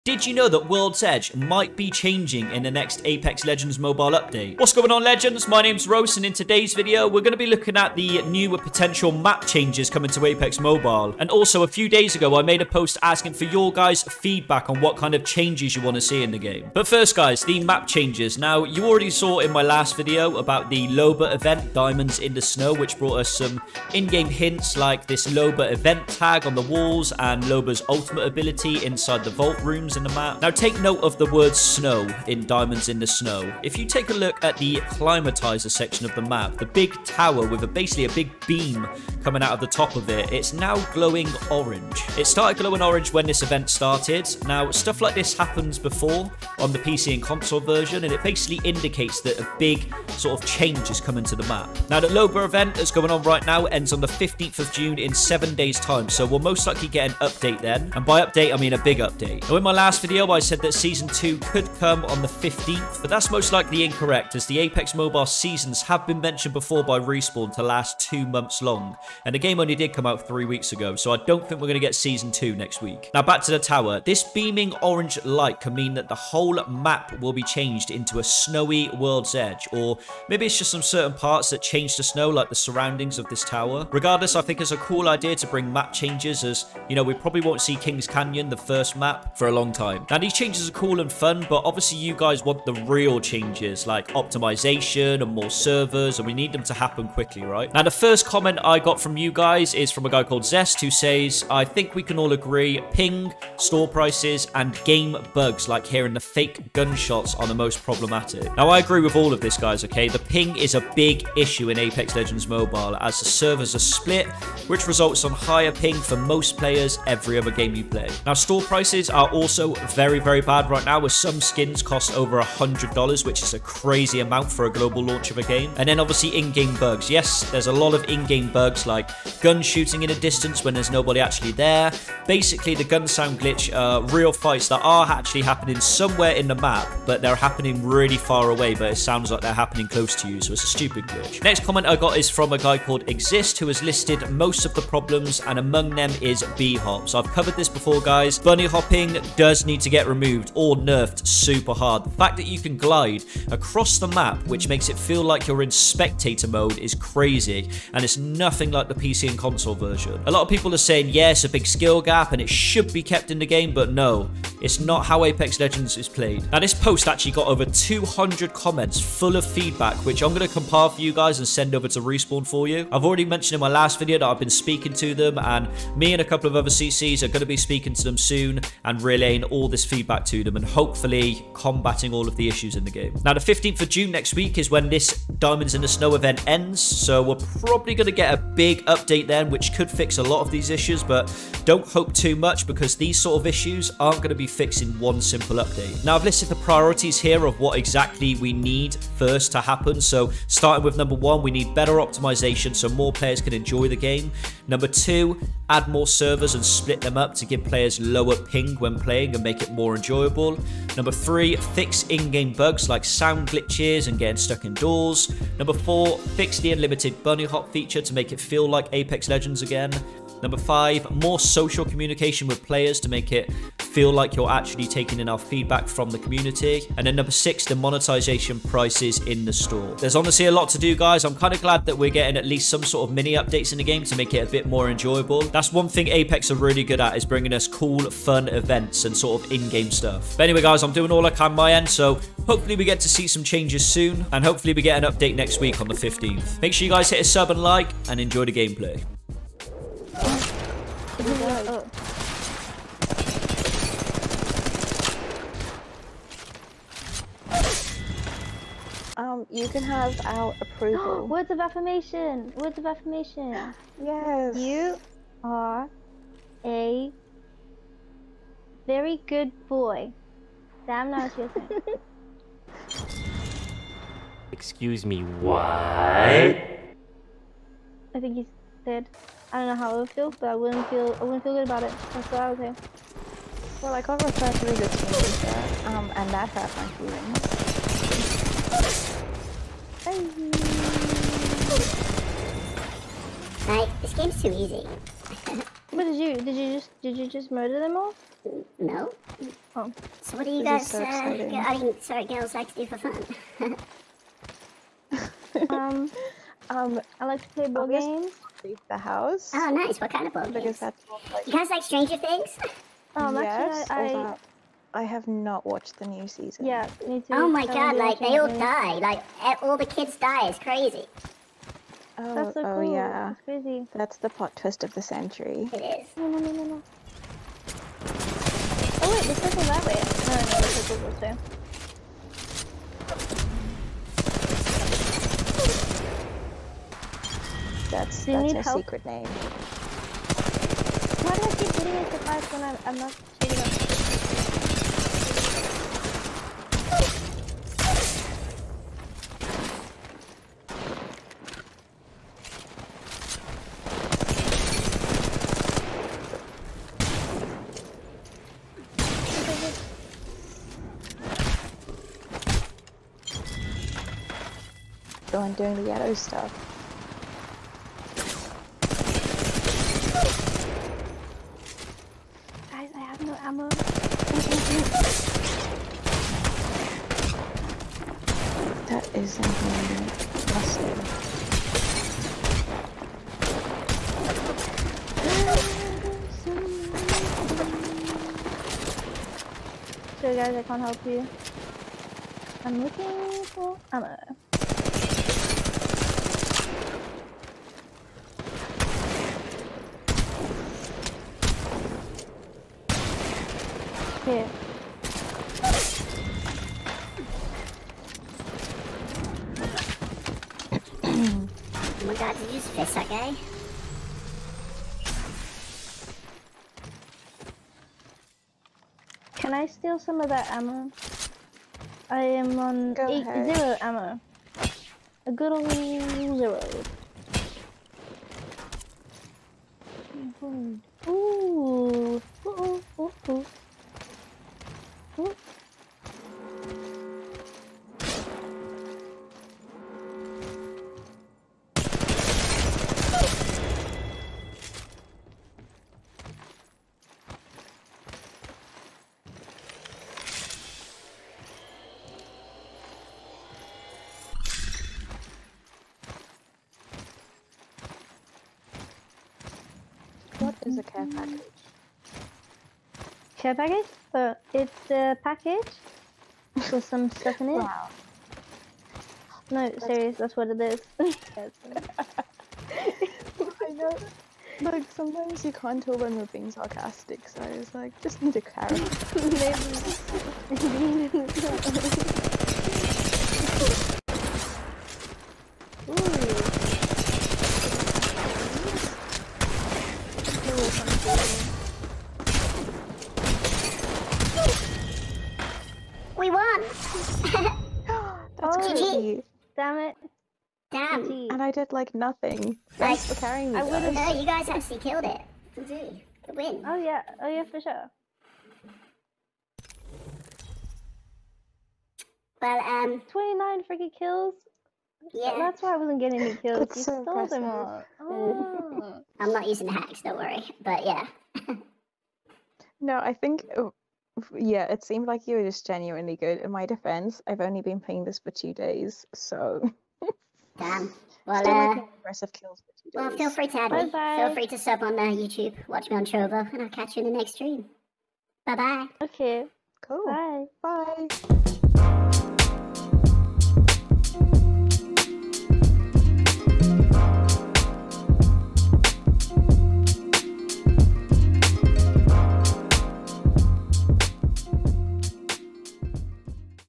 Apex Legends Mobile NEEDS AN UPDATE! (New Map?)
Wassup Legends, in this video we talk about some possible new changes coming to Worlds edge map in Apex Legends Mobile and also what kind of fixes we need for the game including bugs!